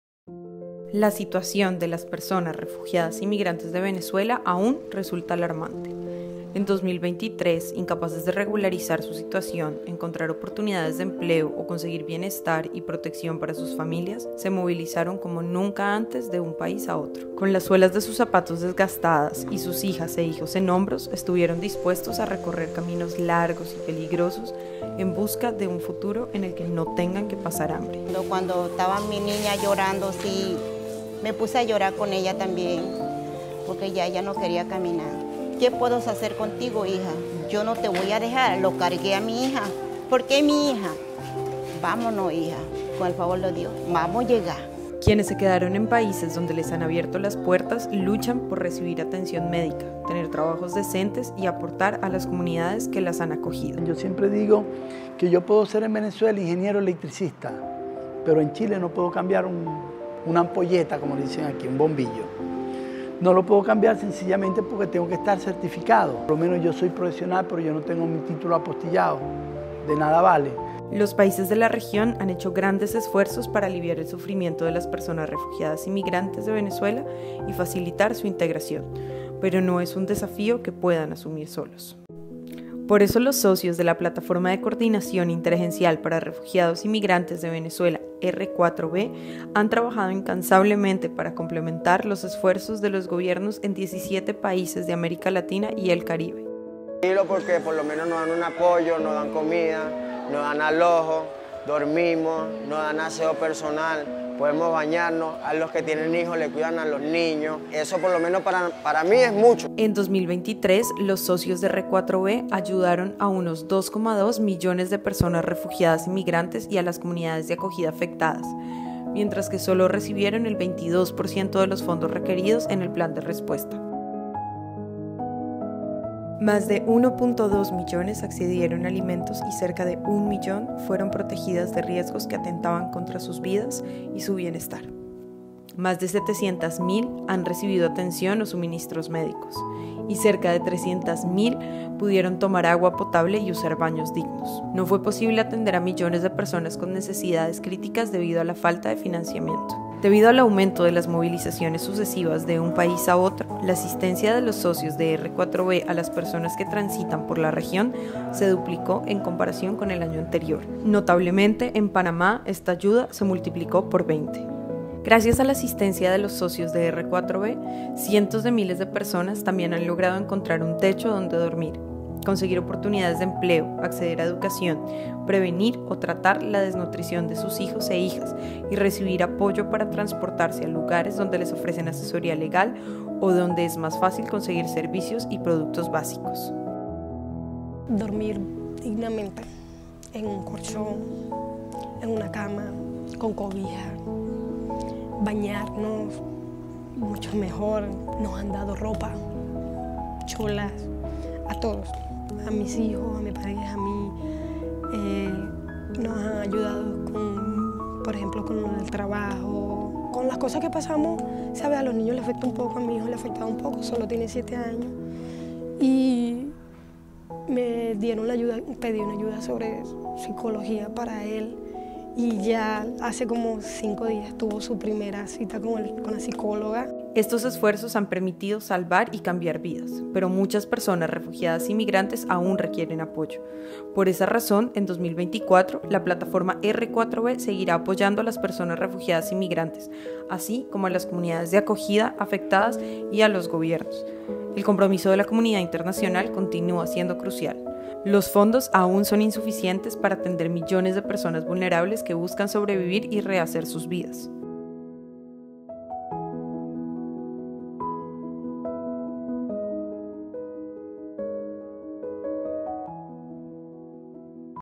Em 2023, salvamos e mudamos vidas (Relatório do fim do ano)
O trabalho dos parceiros da Platforma R4V está salvando e mudando vidas. Cada esforço tem efeitos tangíveis na vida cotidiana de refugiados e migrantes, mas o financiamento para apoiá-los ainda é insuficiente.